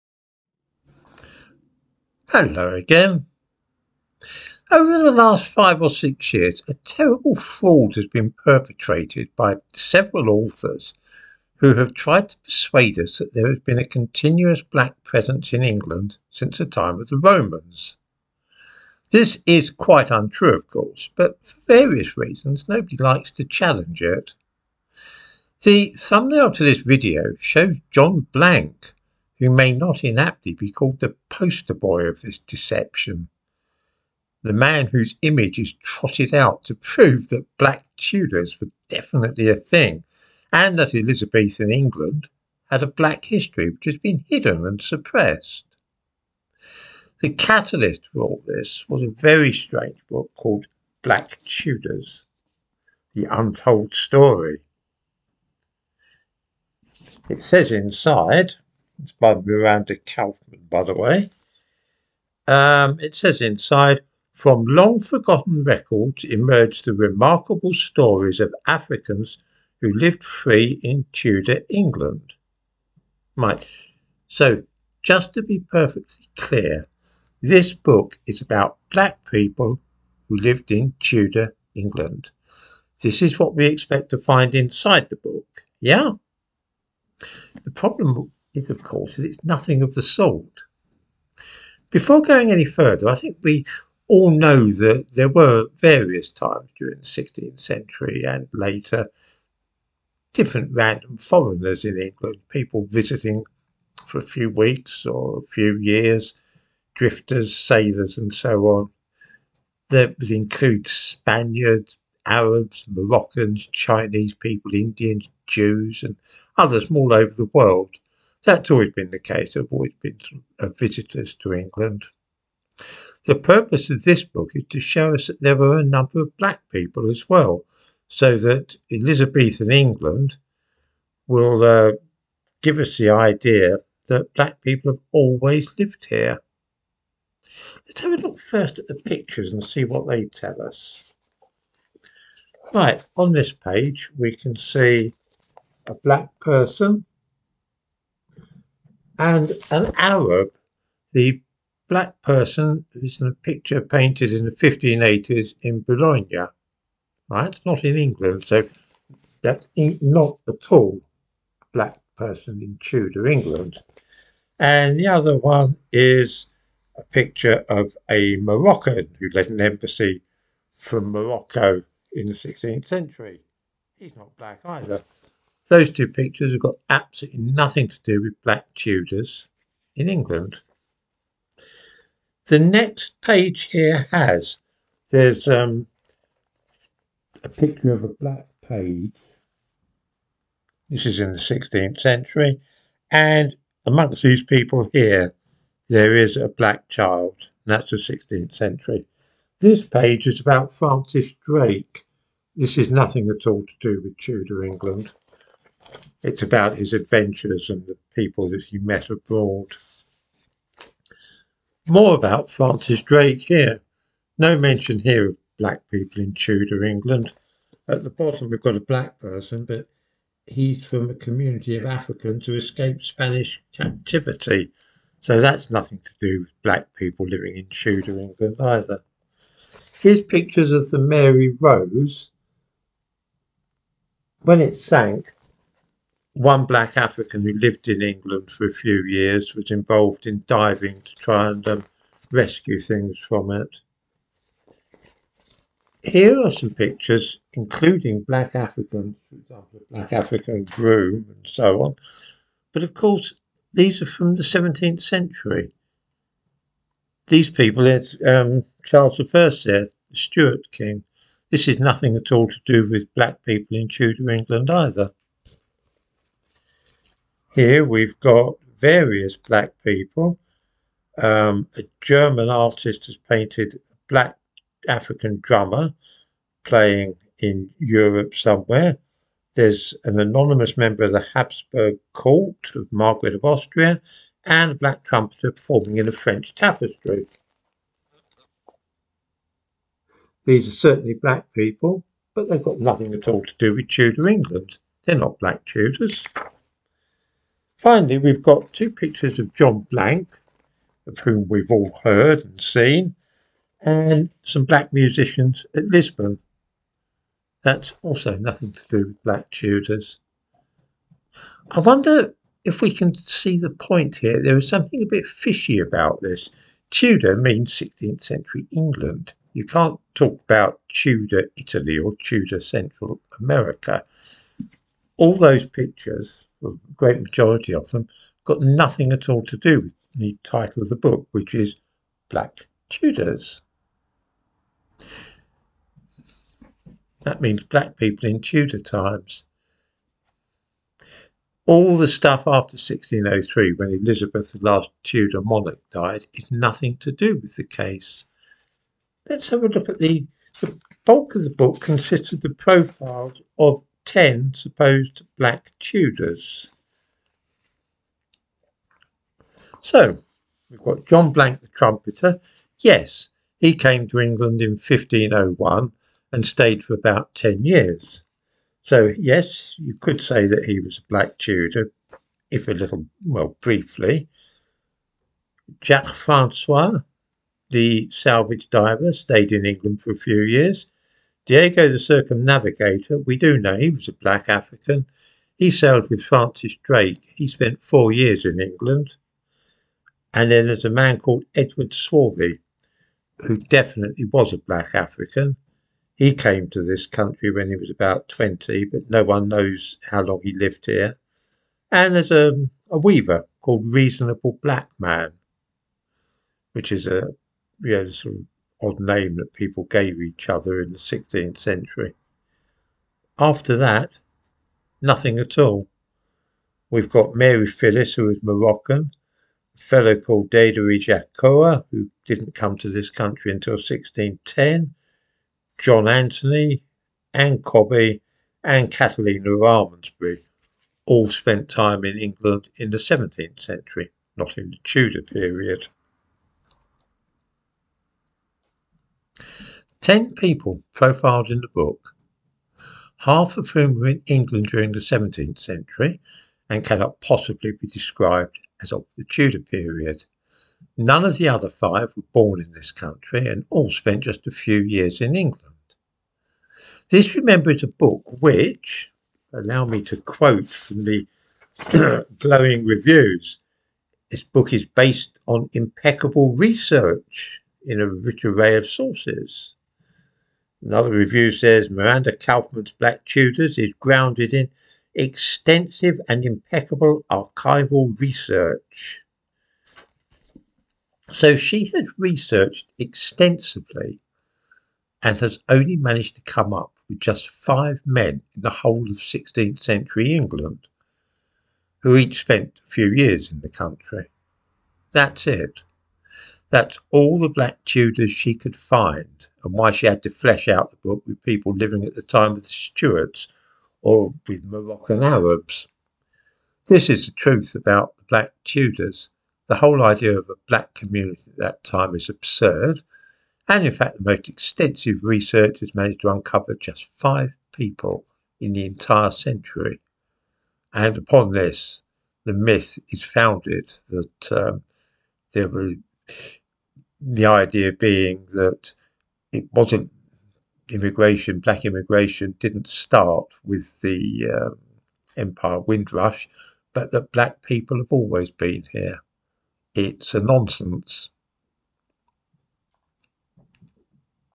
The making of a very modern myth about the supposed black presence in 16th century England
An elaborate false narrative has been constructed and is now taught in schools as history, that a sizeable number of black people lived in this country during the Tudor period.